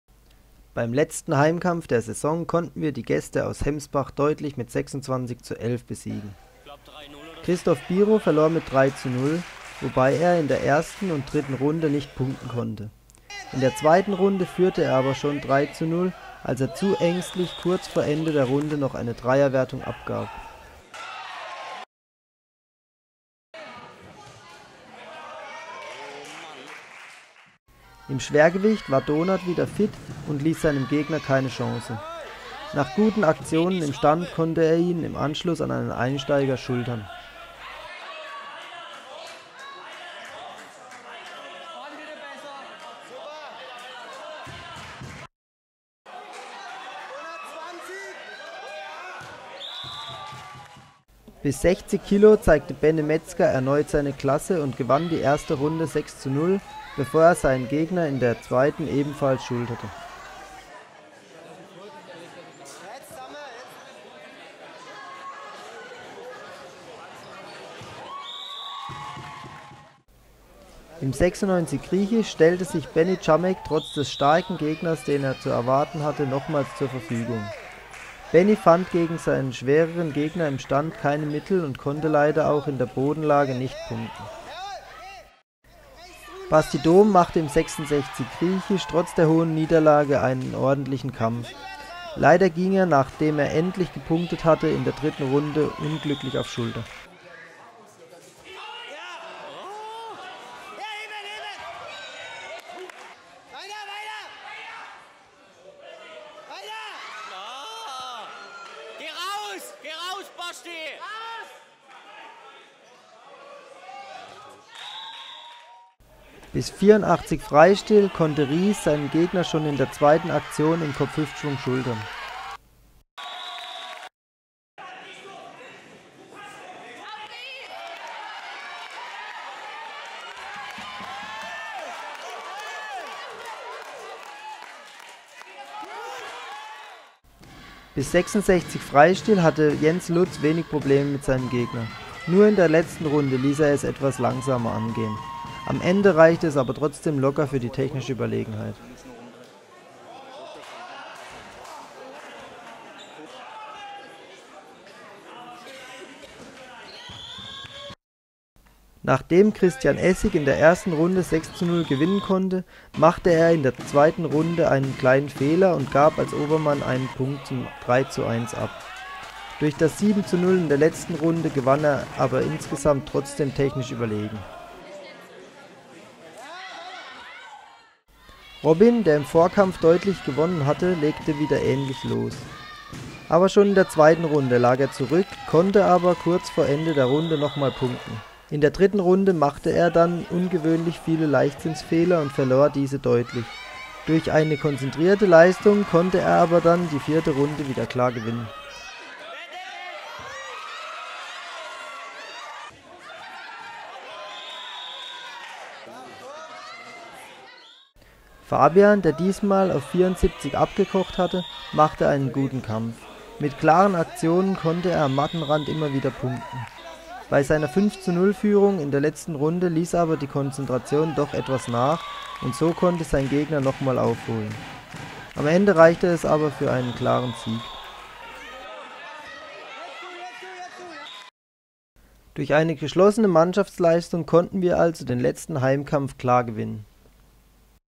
KSV Berghausen - KSV Hemsbach (12.12.09)
Rückrundenkampf in der Oberliga Nordbaden (2009) zwischen dem KSV Berghausen und dem KSV Hemsbach